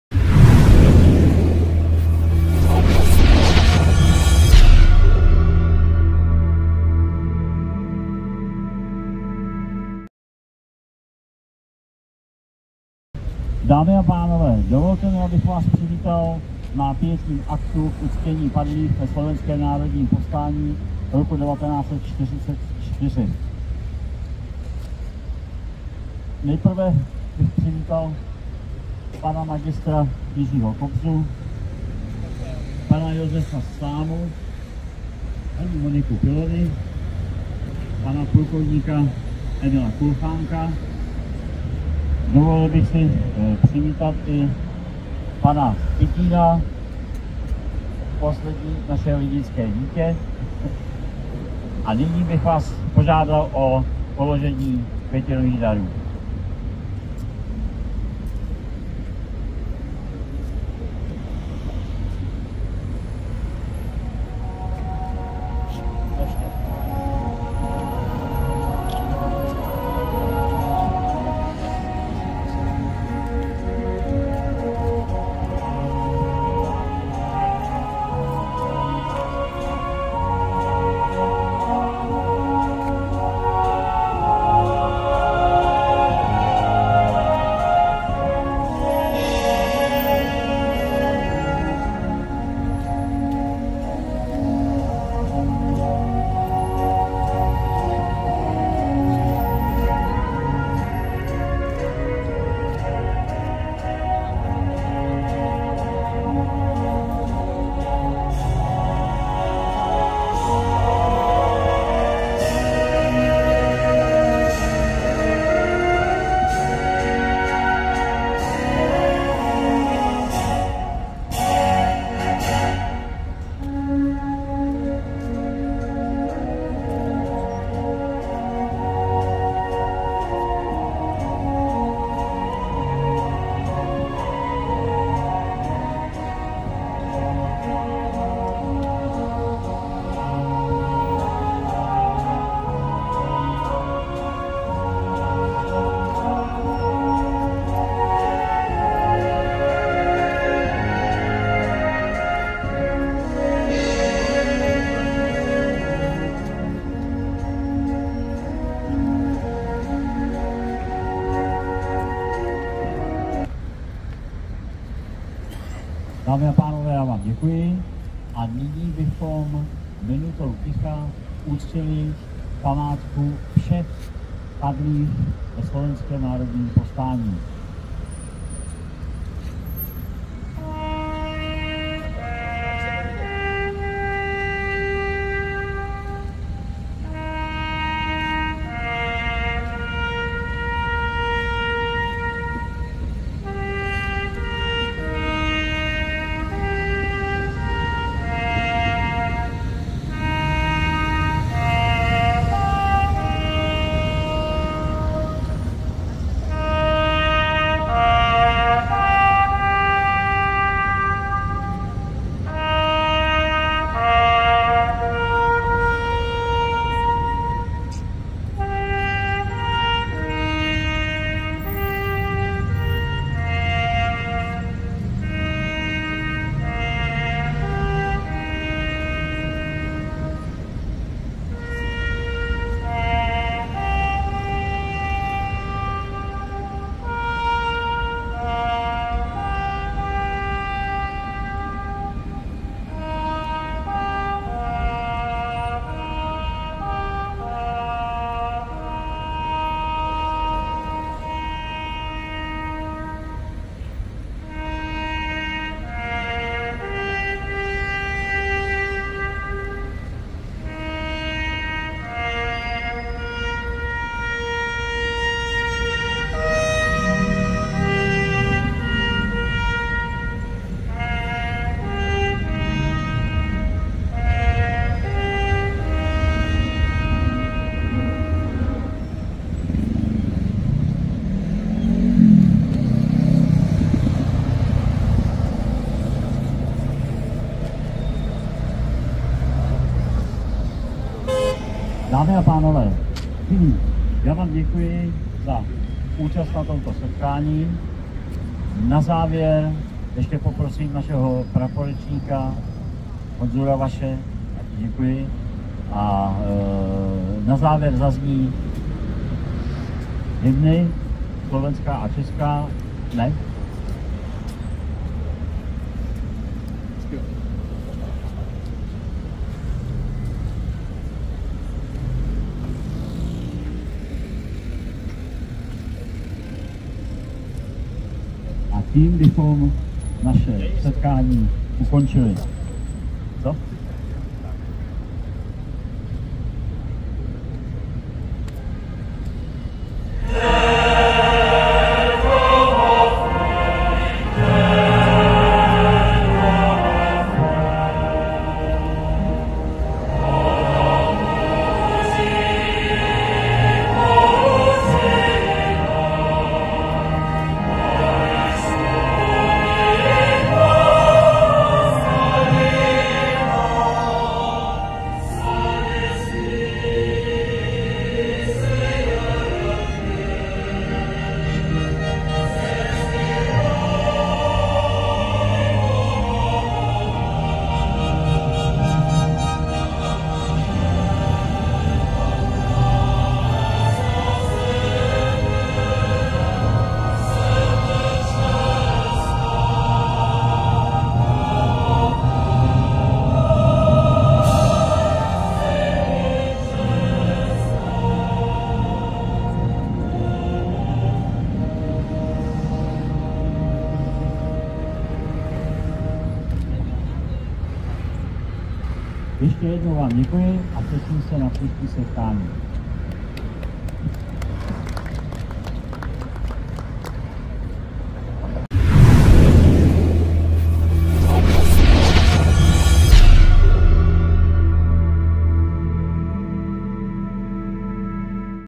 Pietní akt k 80.výročí zahájení Slovenského národního povstání
Vzpomínkový pietní akt u příležitosti 80. výročí zahájení Slovenského národního povstání a k uctění památky všech československých účastníků zahraničního odboje během 2.sv. války 🌹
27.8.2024 Vítězné náměstí, Praha 6 - Dejvice
RaptorTV.cz, kamera Jan Dolens